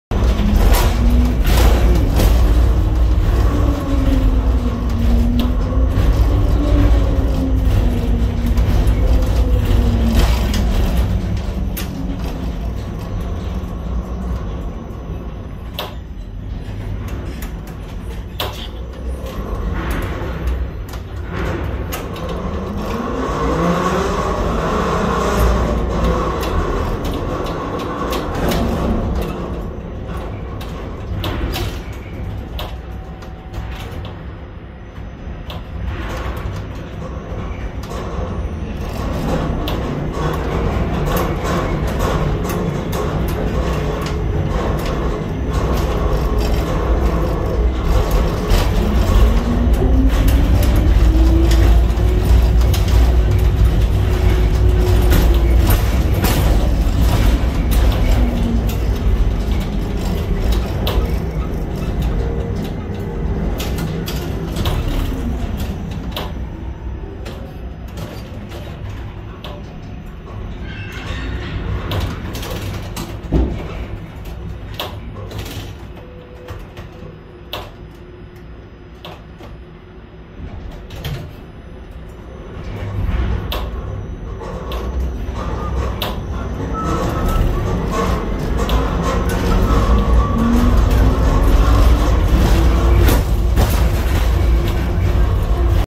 unloading cargo in Port Of LA
unloading containers with hammerhead Gantry crane in Los Angeles